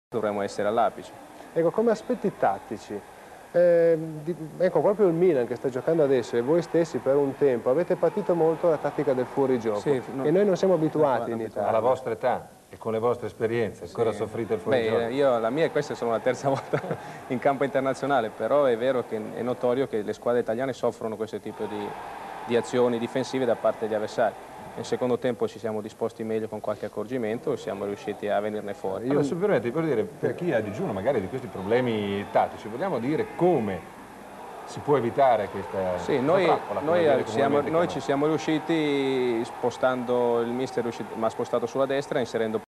Milan-Lokomotiv Lipsia 2-0 Coppa Uefa 85-86 2' Turno A
74’Virdis-76’Hateley